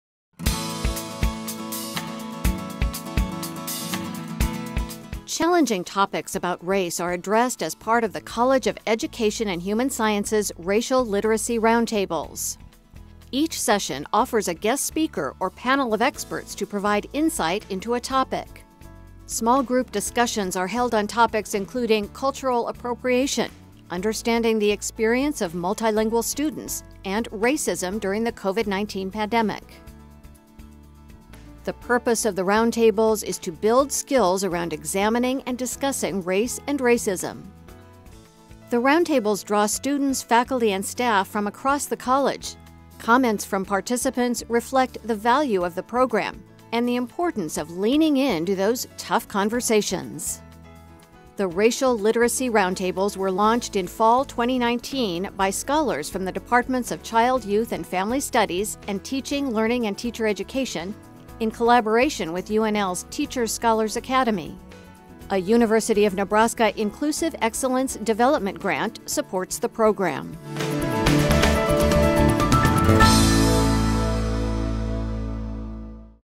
2022 IECA Award -- College of Education and Human Sciences, UNL.
University of Nebraska-Lincoln's College of Education and Human Sciences receives the 2022 Inclusive Excellence Collaboration Award.